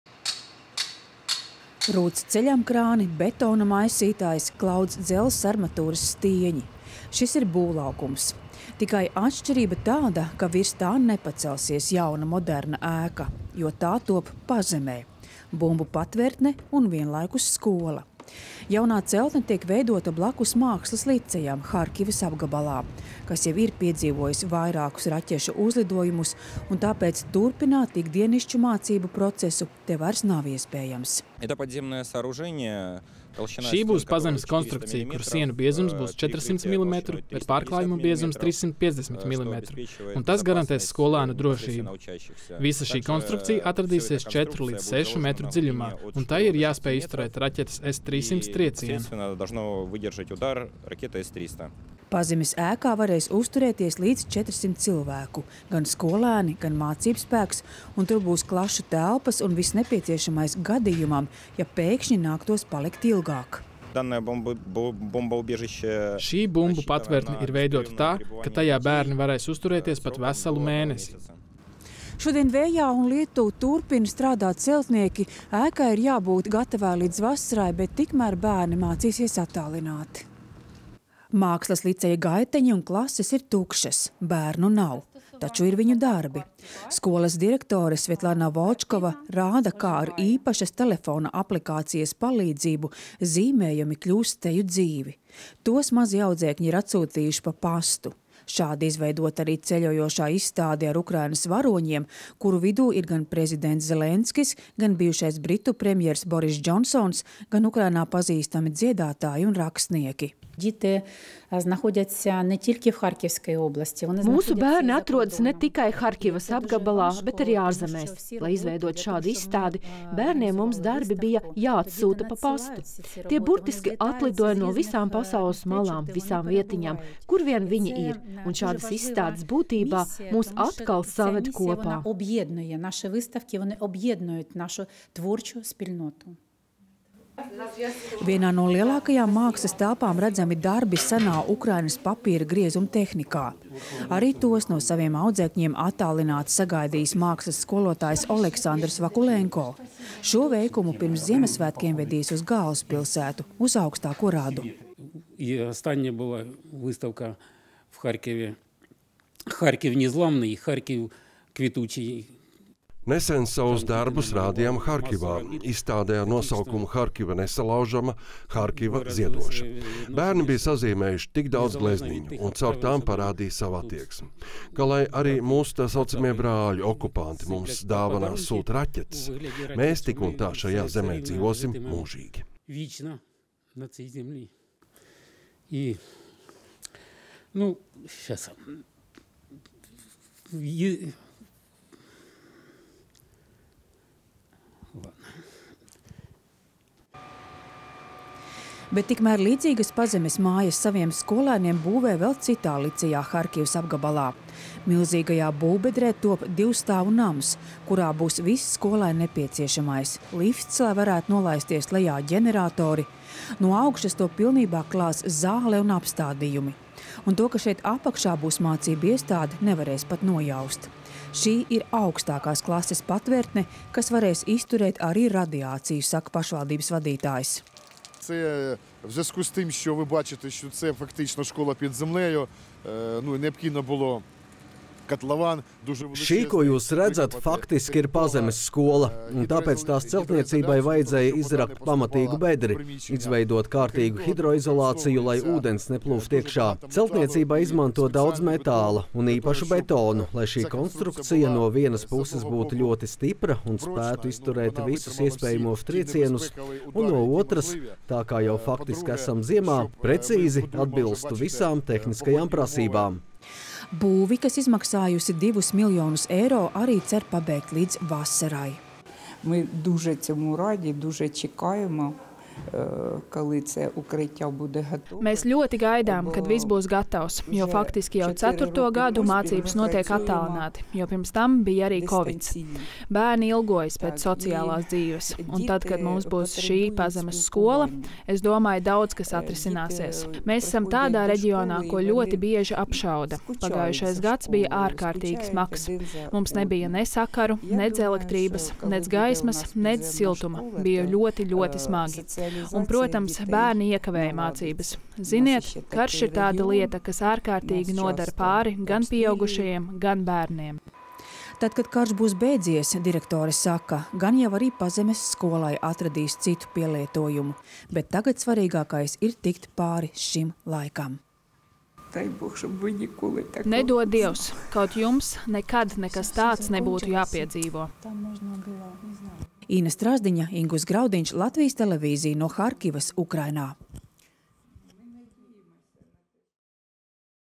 Harkivas apgabalā ceļ pazemes skolas
Lai pasargātos no raķetēm, šāviņiem un vienlaikus ļautu bērniem atkal satikties – ar šādu mērķi Harkivas apgabalā, Ukrainā, ceļ bumbu patvertnes, kas vienlaikus kalpos arī par mācību iestādēm. Harkiva ir viens no reģioniem Ukrainā, ko Krievijas armija apšauda intensīvi, tādēļ mācības klātienē notikt nevar. Taču jaunās, drošās būves dod cerību, ka, arī karam ieilgstot, bērniem būs iespēja kaut daļēji atgriezties skolas solā.